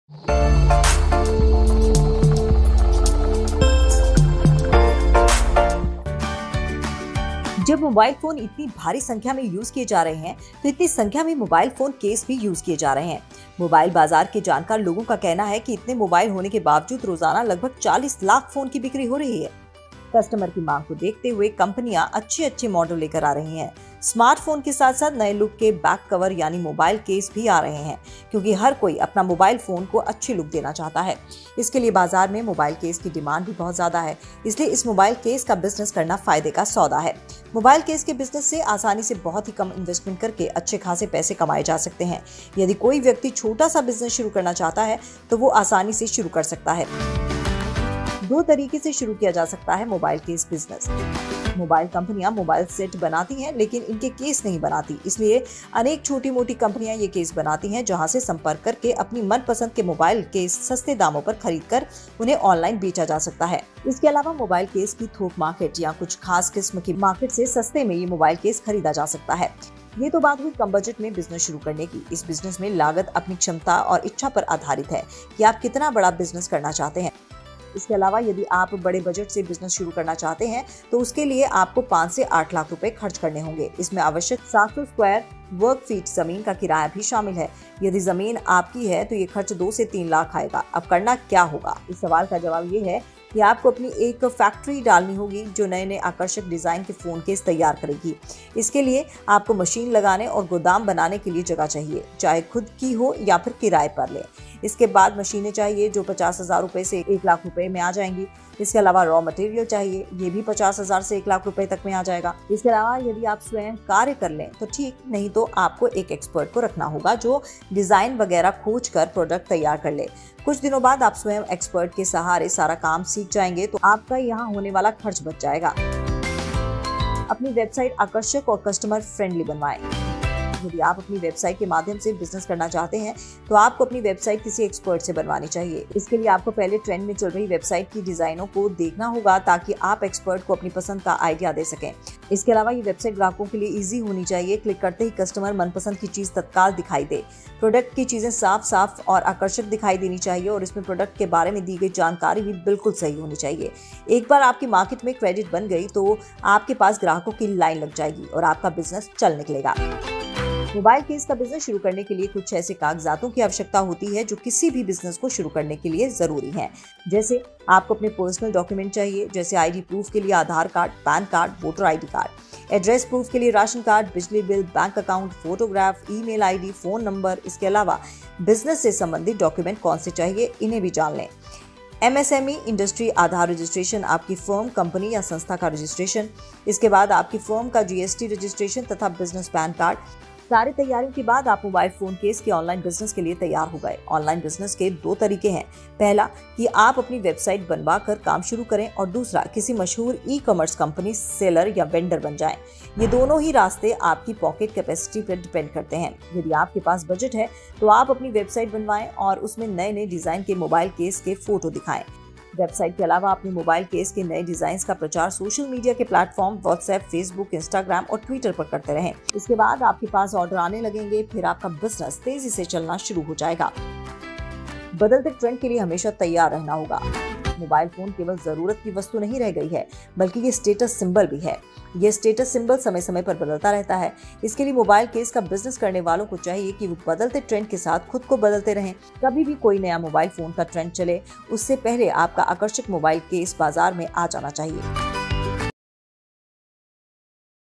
How to Start Phone Case Business? | Phone case बिज़नेस करके लाखो कमाओ | Vyapar Udhyog
A Phone Case Business is a profitable way to make money. The market for phone cases was worth $10 billion in 2020 and is expected to grow 7.5% each year until 2025. That means there will be a lot of money to be made if you know how to build a successful phone case business? Then watch this video till the end